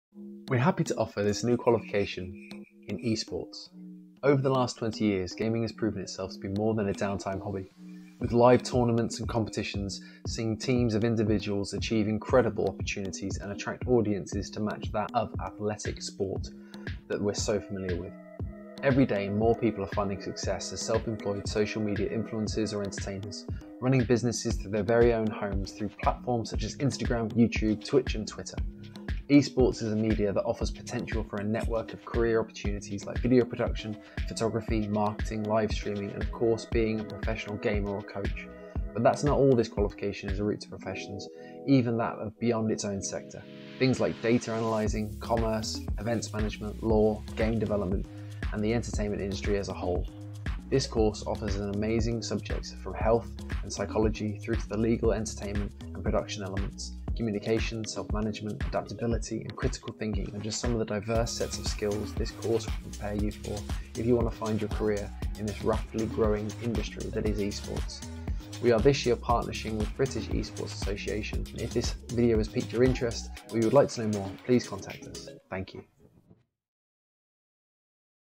Discover Esports at CRC
Study Level 3 Vocational Course in Esports at Cambridge Regional College

This course is designed to give you a thorough introduction to Esports as well as new evolving online media. At present over 50% of the UK is gaming and there is an ever-increasing industry that is emerging as a result, short form content and live streaming are replacing traditional media content.

or register for one of our Open Days